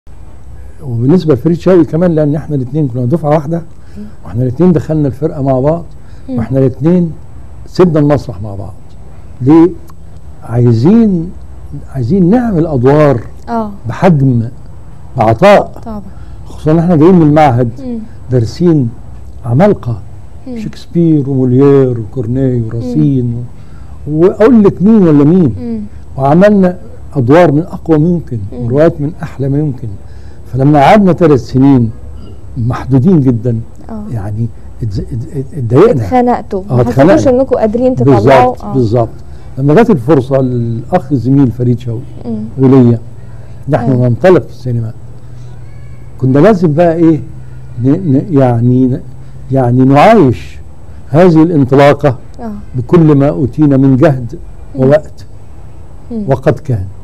السر الحقيقي لابتعاد شكري سرحان وفريد شوقي عن المسرح
FOLLOW US ON :
ـــــــــــــــــــــــــــــــــــــ